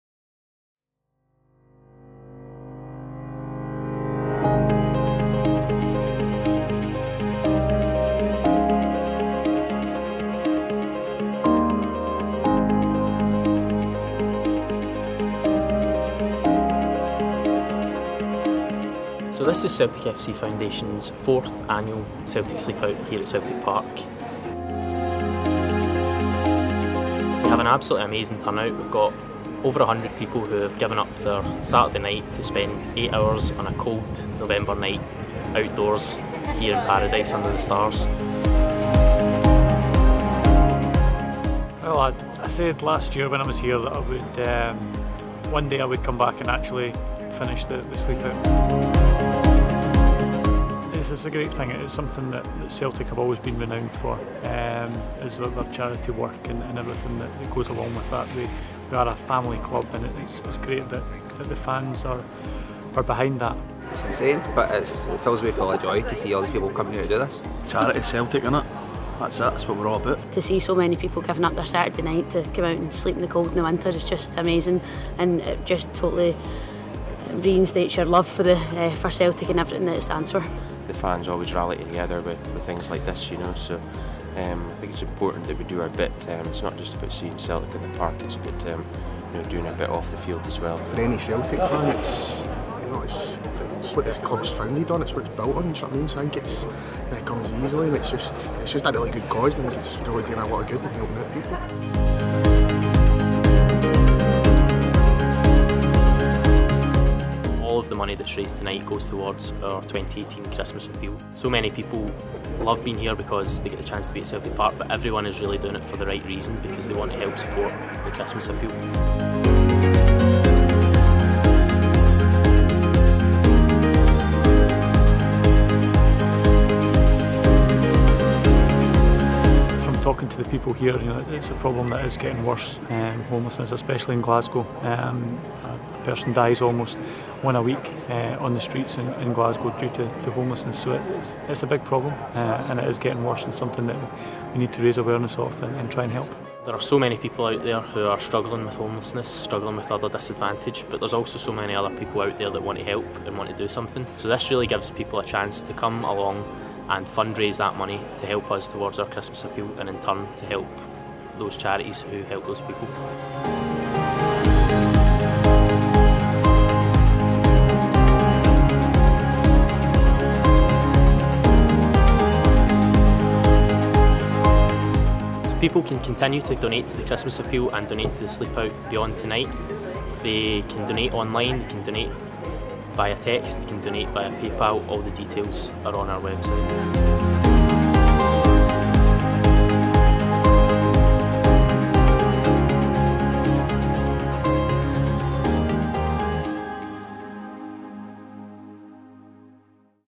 Celtic FC - Foundation Sleep Out 2018
😴 Last weekend, as part of our Christmas Appeal we hosted our 'Sleep Out' event in Paradise

All eyes now turn to tonight’s ‘Sleep Out’ in London 👀